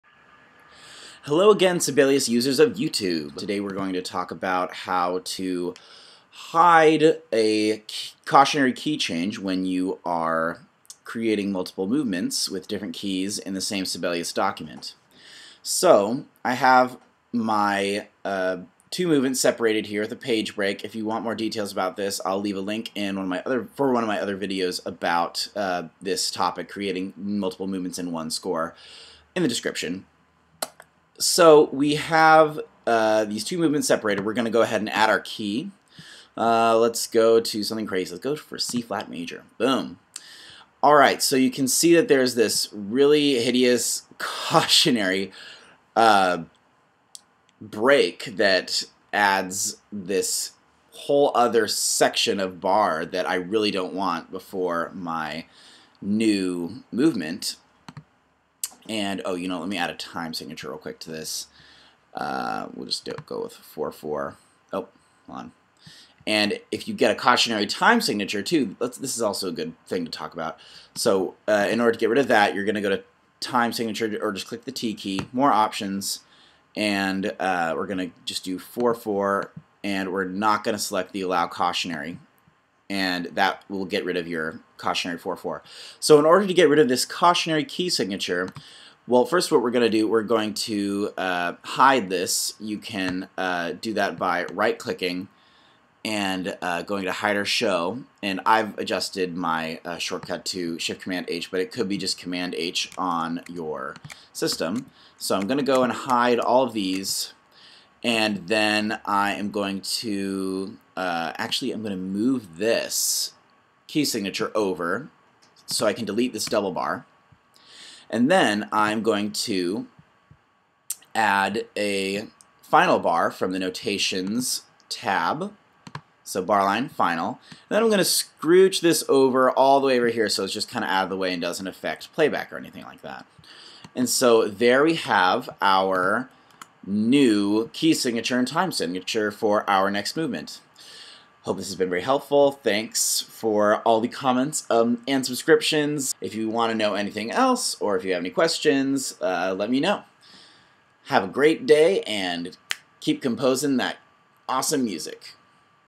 HIDE "CAUTIONARY" KEY SIGNATURE | Sibelius Tutorial | Composer's Guide
This tutorial covers how to hide a cautionary key and cautionary key space at the end of a movement when you make multiple movement pieces in a single Sibelius document. These tools work for any version of Sibelius 7 or newer.

For more info on creating multiple movements in a single .sib file, check out this TUT: